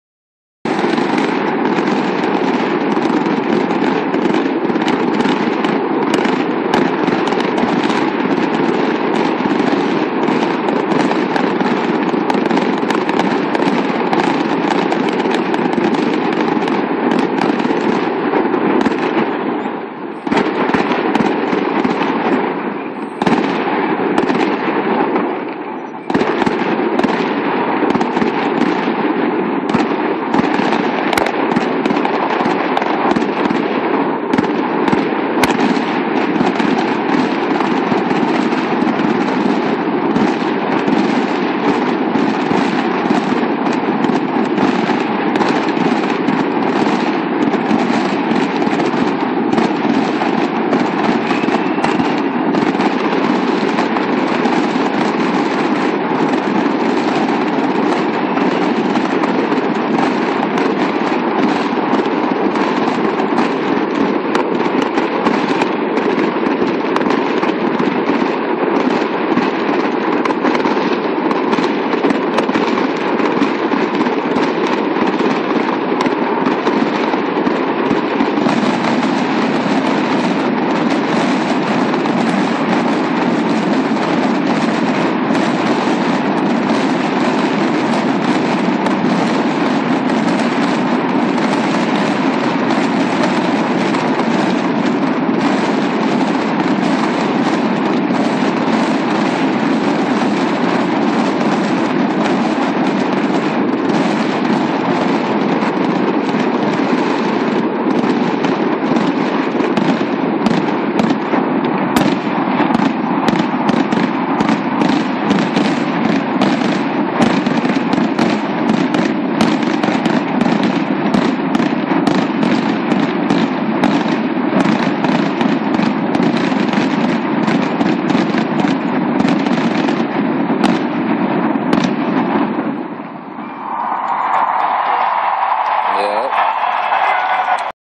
Canada Day 2011
On Dad's balcony... View of the fireworks from Mississauga City Center